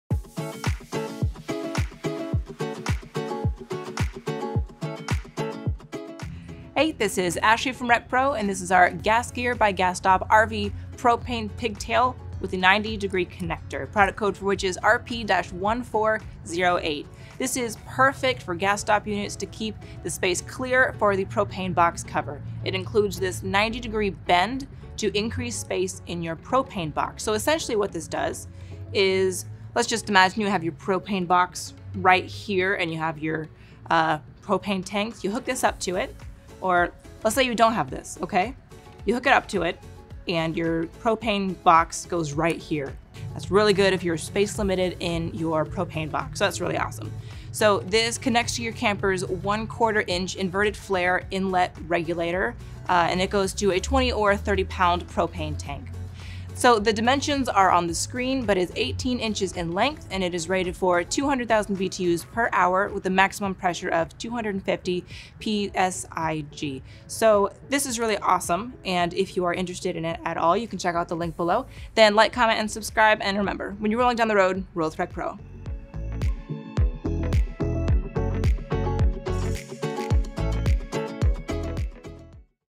GasGear By GasStop RV Propane Pigtail with 90 Connector
Propane gas is probably the most common source of energy on an RV or trailer, running a variety of items as well as serving as a potential backup energy source. Because of this, it's important to keep your RV propane system working well and keep all of the items involved in doing so in good shape. On a smaller RV or trailer or on a towable unit, the propane tanks are often in 20lb or 30lb tanks, many times under a cover. Using a GasStop device can greatly improve the safety of such a system, however, there are issues that can occur with the amount of space available inside of the cover area. When you have a smaller amount of space within your propane tank storage bay, adding another propane accessory can cause the pigtail hose connecting the LP tank to the LP system to brush against the cover. This could lead to wear and tear and can even prevent the cover from fitting properly or at all. Luckily, however, there is an easy solution to this problem that not only saves you space but keeps your propane system working well.